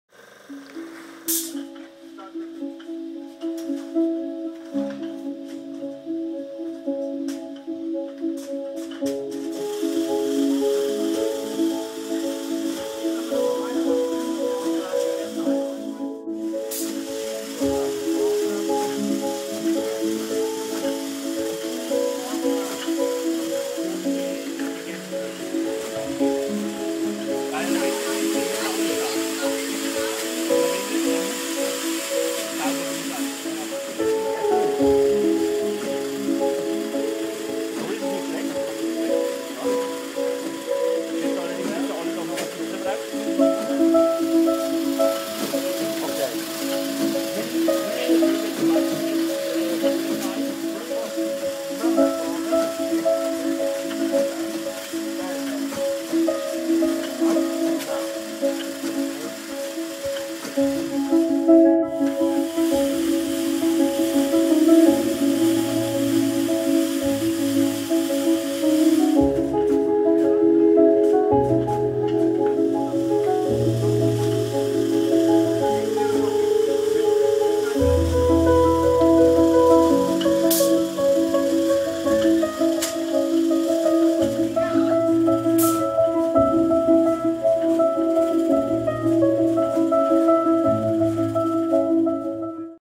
RAP Carosel
Macchina per potare piante in vaso, ideale per vasi da diametro 10 a diametro 18.